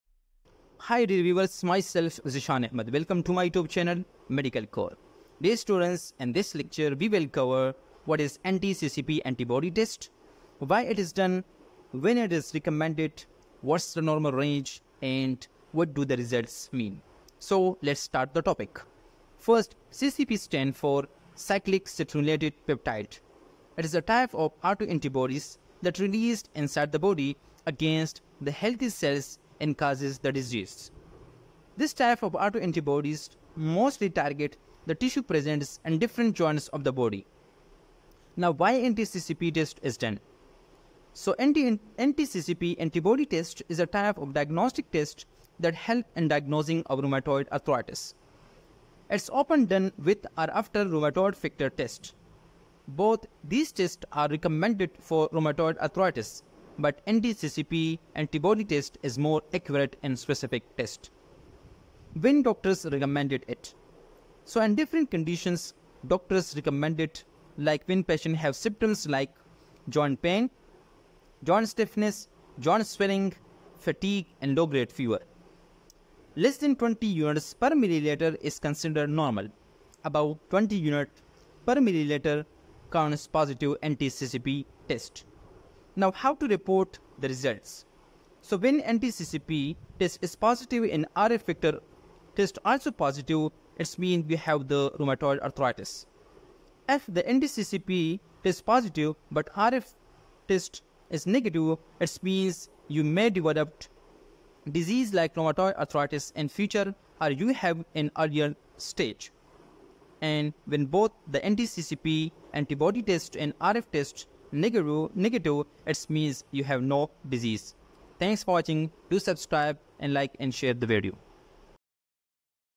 Anti- CCP Antibody blood Test |When it's Recommended | Why it's Done| Normal Range | medical core
Dear Students, anti - CCP antibody Test  is a type of confirmatory lab test that's done for Rheumatoid Arthritis.
Why doctors usually recommended this test after RF test. When it's Recommended. What's the normal range for anti ccp antibody. All these things you will seek in this short video.
So keep watch till the end.
Thanks ❤️